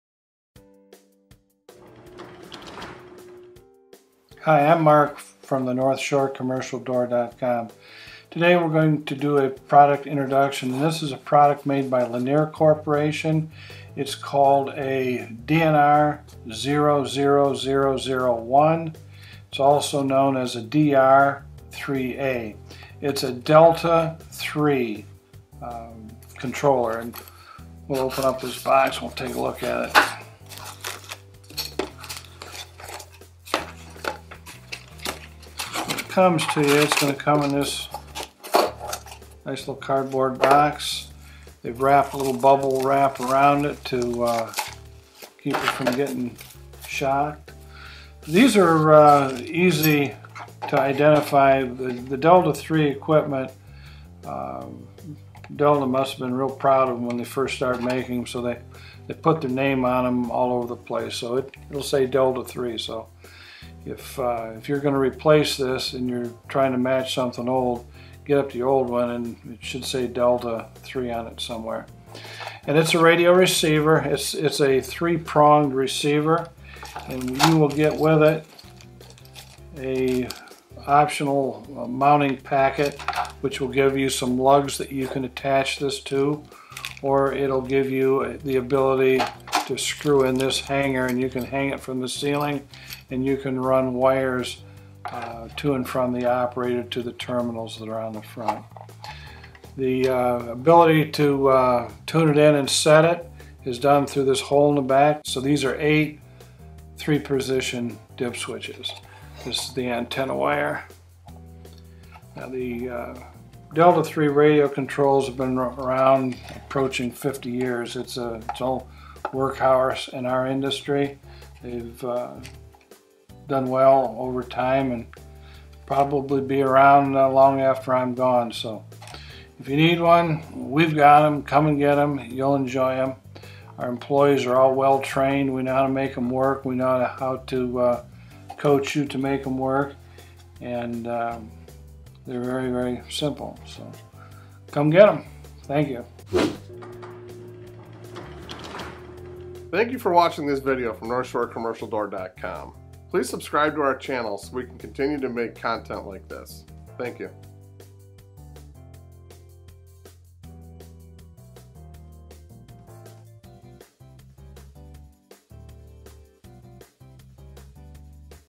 Linear Delta 3 DNR00001 DR DR3A Single Channel Gate or Garage Door Receiver
When you need to operate a security access point with a compact, easy-to-program device, the Linear Delta-3 DNR00001 DR DR3A single-channel gate or garage door receiver might be perfect for your needs. With a wire lead antenna and a mounting bracket, this ultra-small, 3-wire garage door receiver is easy to install and ready to go right out of the box. It's available in 12-volt and 24-volt models.

The Linear Delta-3 DNR00001 DR DR3A single-channel gate or garage door receiver activates one device, whether it's a security gate or a garage door, by operating on a 310MHz frequency. It measures just 5.5 inches across, 4 inches high and 1.25 inches wide, so it doesn't take up much room when properly mounted to a wall.
Comes with wire lead antenna and mounting bracket
Operates at 310MHz frequency
256 possible codes set by dip switches
Dimensions: 5.5 inches wide, 4 inches high and 1.25 inches wide